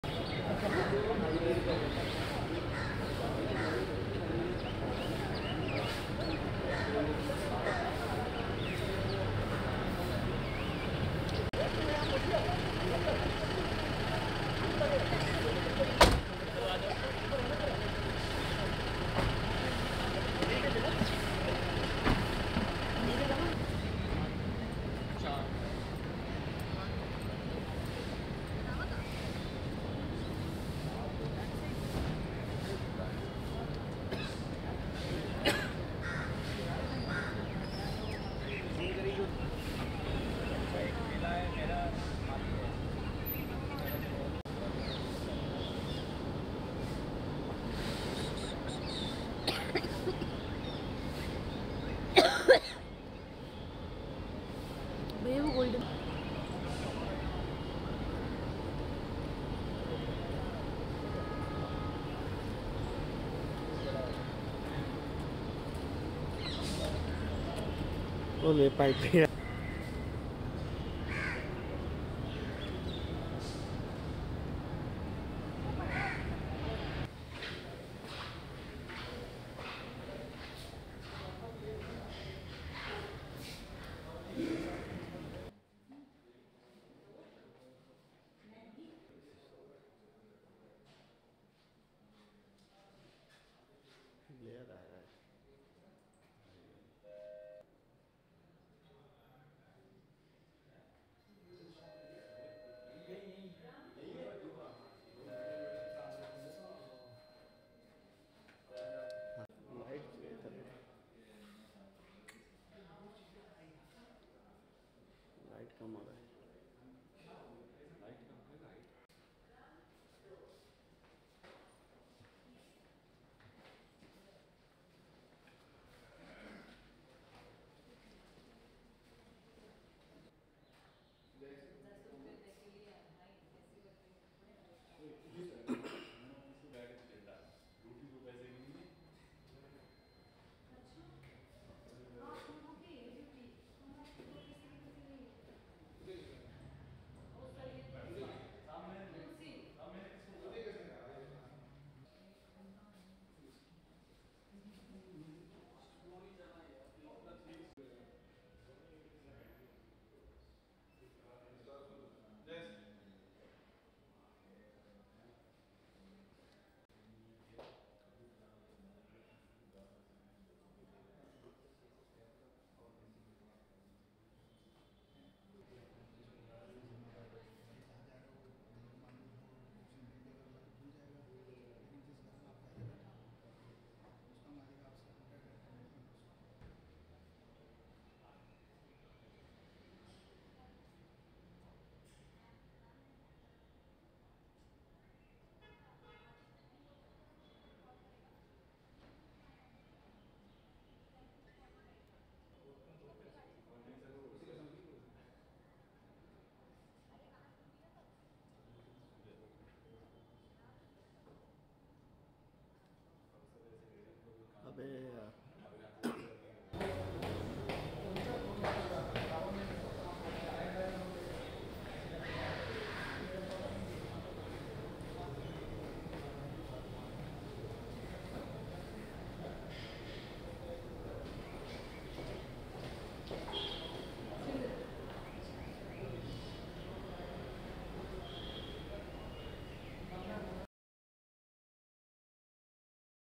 CISF Headquarters in CGO Complex, Delhi
A walk inside the headquarters of Central Industrial Security Force of India in New Delhi. This headquarters has recently gone through renovation and now it has all the modern amenities which a corporate office has. This headquarters building is located in 13th Block of CGO Complex in Lodhi Road, Delhi.